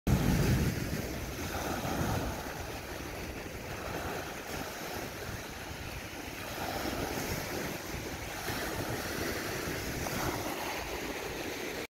Waves (cameo by Mr. Bill)
Light surf Mr. Bill greyhound retiree ex-racer rescue